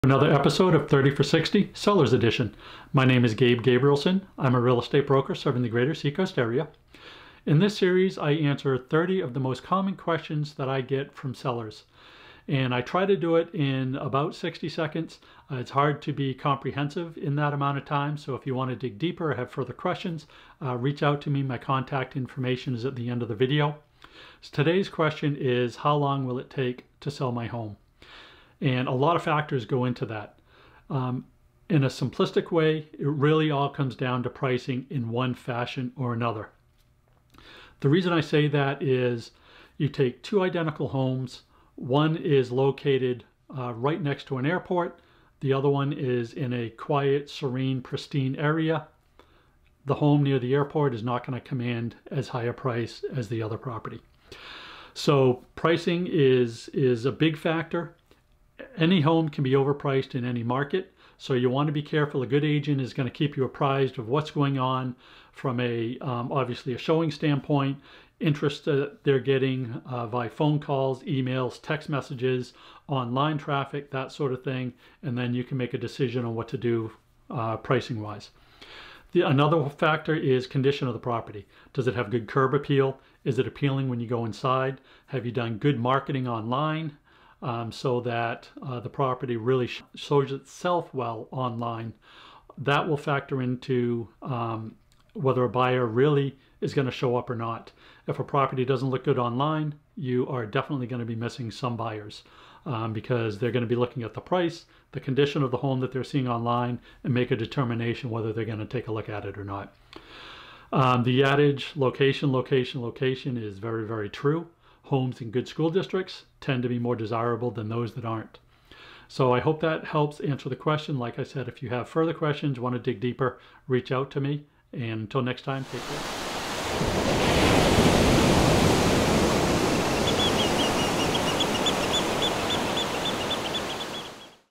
How Long Will It Take To Sell My Home?
How long will it take to sell my home? The million dollar question that is multi-faceted. Watch to find out more.